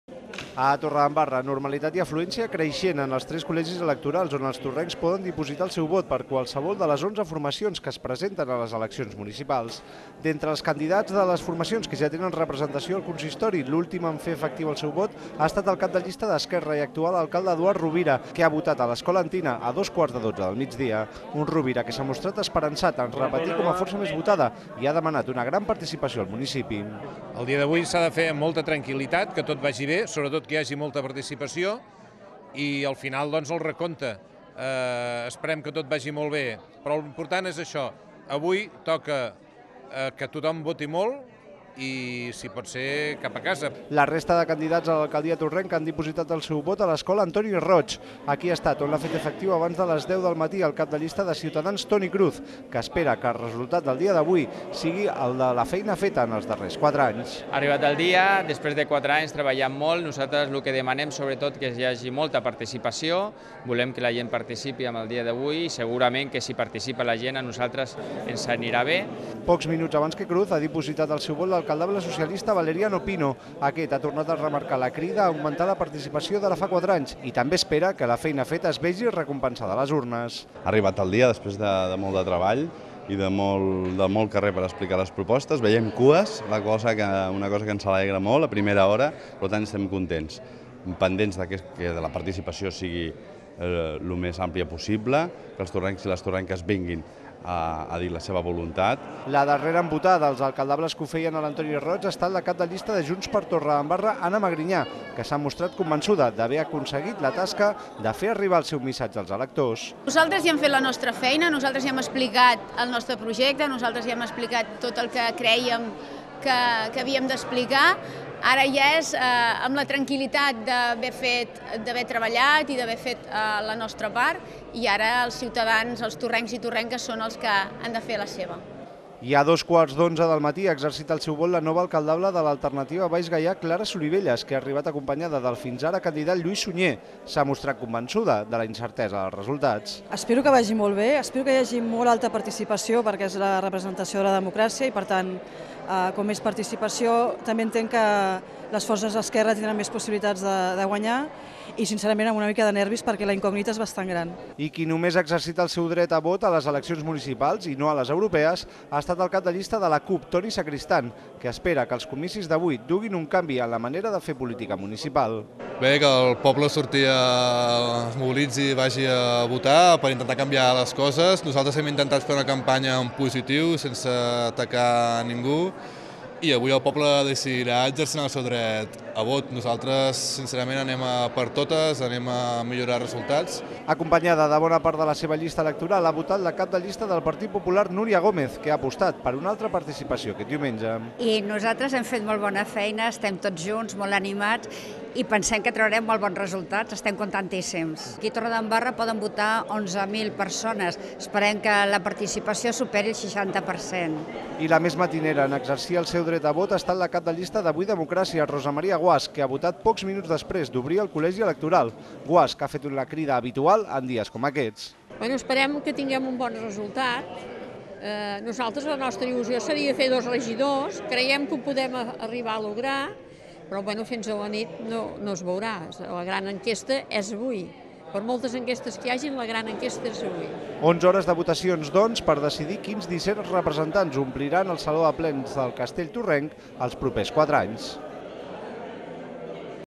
Els torrencs trien entre onze formacions a les municipals
El 26-M atorgarà a Torredembarra disset cadires al Saló de Plens torrenc a les que opten fins a onze formacions. Els caps de llsta dels partits que ja tenen representació han votat entre les nou i dos quarts de dotze del matí